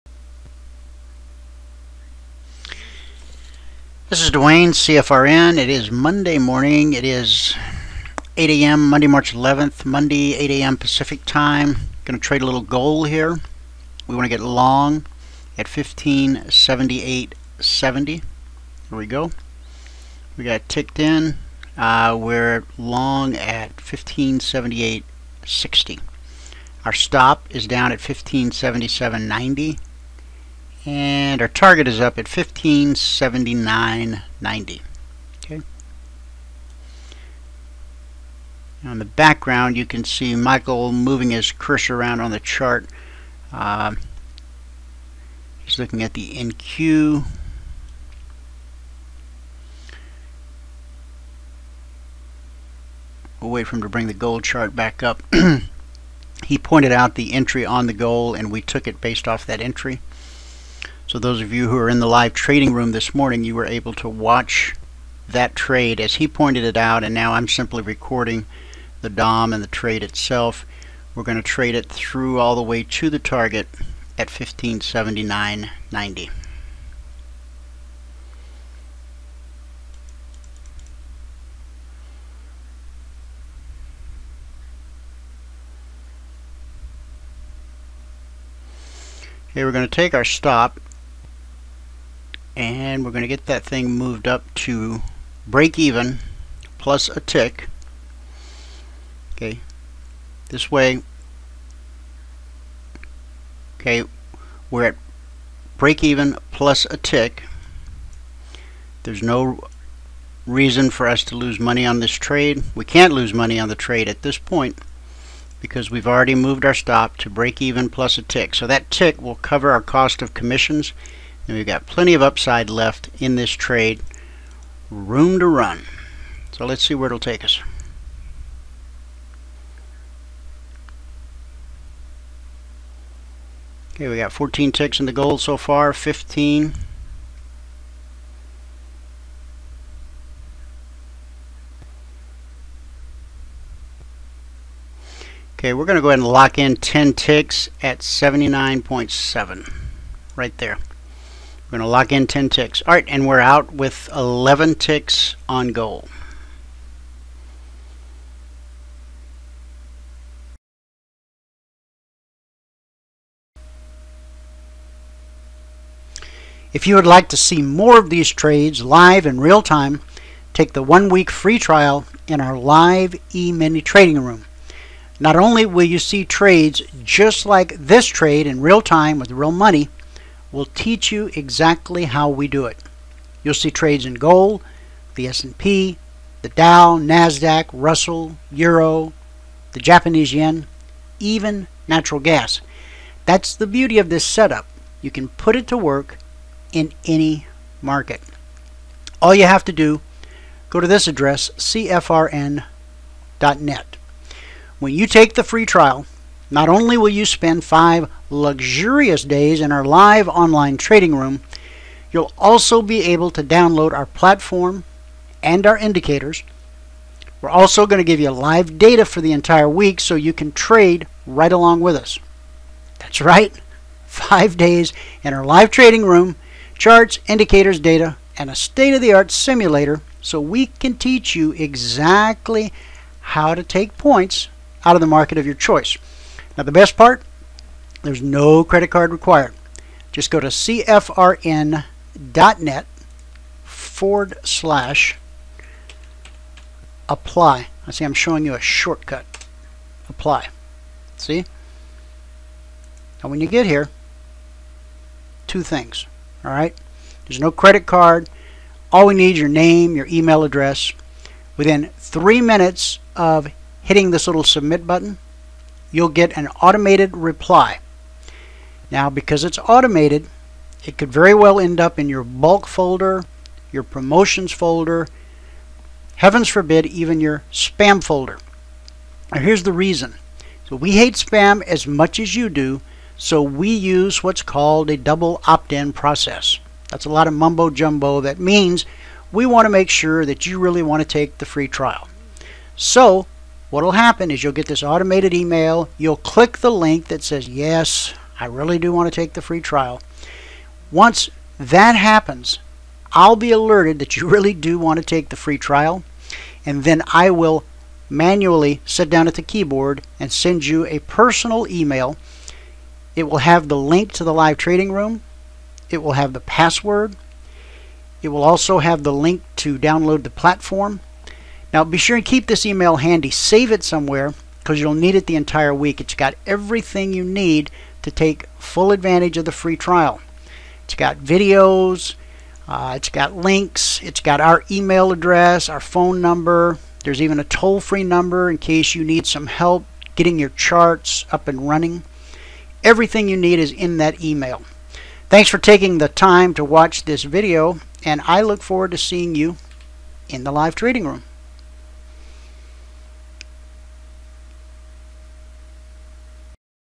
CFRN Gold Trade - Our Daily Goal in 5 Minutes
In this short video we use our Slingshot Trade to take 11 ticks out of Gold. We teach our Partners to set a daily daytrading goal of "2 ES Points" or "$100 per contract" if trading a different market. In this video we accomplish our daily goal in 5 minutes.